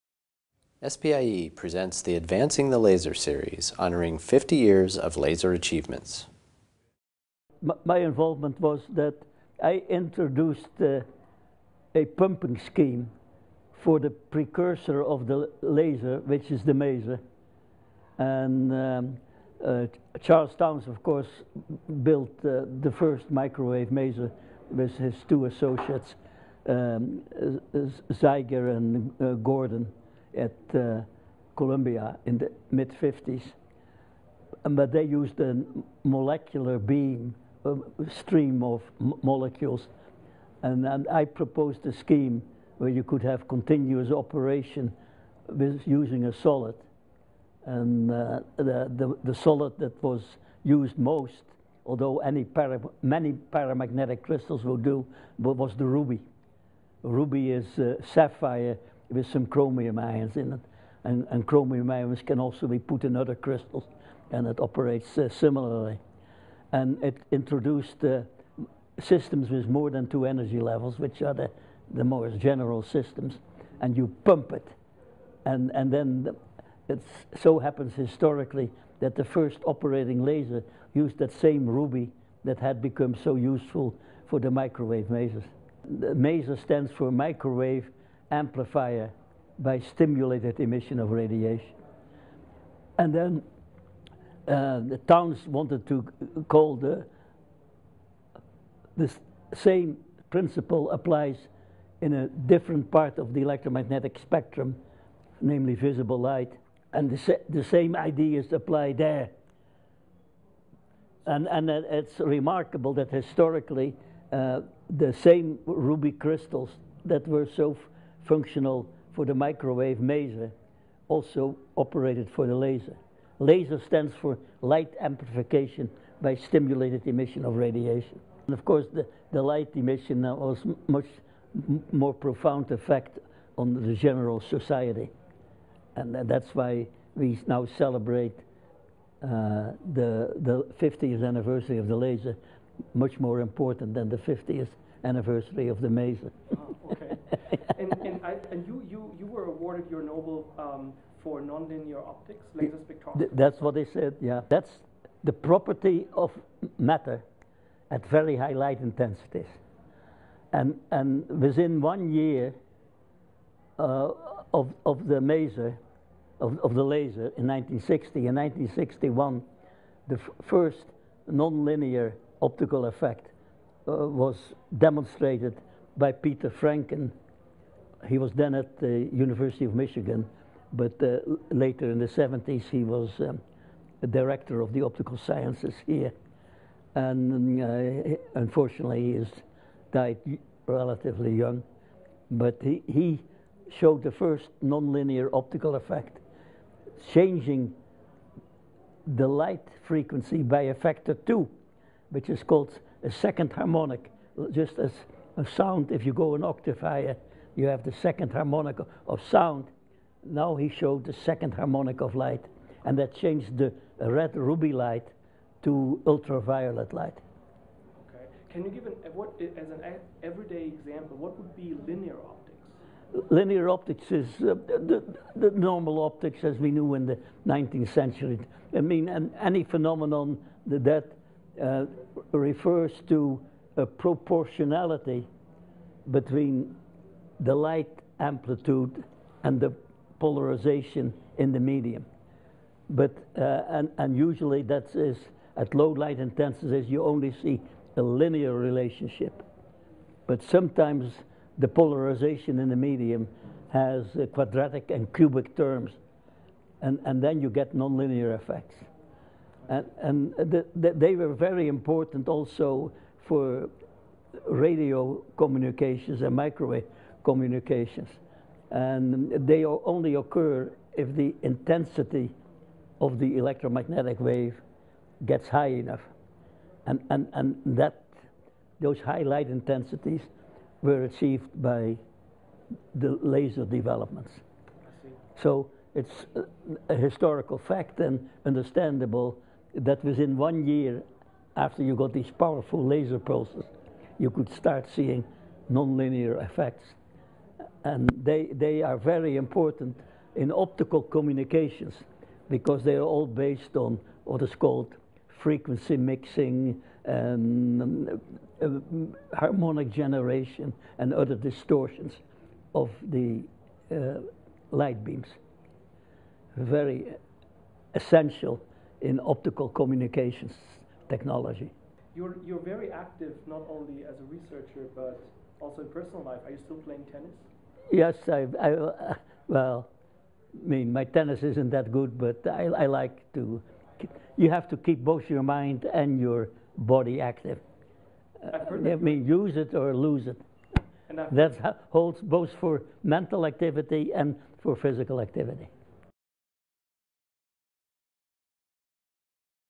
Video interview with Nicolaas Bloembergen
Nicolaas Bloembergen celebrated his 90th birthday in March 2010 with a scientific symposium and reception at the University of Arizona College of Optical Sciences, attended by three other Nobel laureates: Roy J. Glauber, John L. Hall, and Charles H. Townes.

Bloembergen received the Nobel Prize in 1981 for his contributions to the field of nonlinear optics and to the development of laser spectroscopy. He was a corecipient with Arthur Schawlow of the United States and Kai Manne Borje Siegbahn of Sweden of the 1981 Nobel Prize for Physics for their revolutionary spectroscopic studies of the interaction of electromagnetic radiation with matter. Bloembergen made a pioneering use of lasers in these investigations. His research has included nuclear and electronic magnetic resonance, solid state masers and lasers, and nonlinear optics and spectroscopy. His work on proton spin relaxation times in water and aqueous solutions, carried out in 1946 and 1947 under the guidance of his PhD thesis advisor, Edward M. Purcell, later became the basis for the medical diagnostic technique of magnetic resonance imaging (MRI).

In his autobiography on the Nobel Prize website, Bloembergen stated, "I held the opinion -- even after Schawlow and Townes published their proposal for an optical maser in 1958 -- that it would be impossible for a small academic laboratory, without previous expertise in optics, to compete successfully in the realization of lasers. This may have been a self-fulfilling prophesy, but it is a matter of record that nearly all types of lasers were first reduced to practice in industrial laboratories, predominantly in the U.S.A."

Bloembergen first came to the United States in 1945, after spending World War II "hiding indoors from the Nazis, eating tulip bulbs to fill the stomach and reading Kramers' book Quantum Theorie des Elektrons und der Strahlung by the light of a storm lamp." He received his PhD at the University of Leiden in 1948, after doing research at Harvard, then finally emigrated to the U.S. (and returned to Harvard) in 1949. He and his wife Deli moved to Arizona, and he has been on the UA College of Optical Sciences faculty since 2001.

He was interviewed at his birthday celebration by Daniel Stolte of the UA University Communications office, and this video is courtesy of the University of Arizona.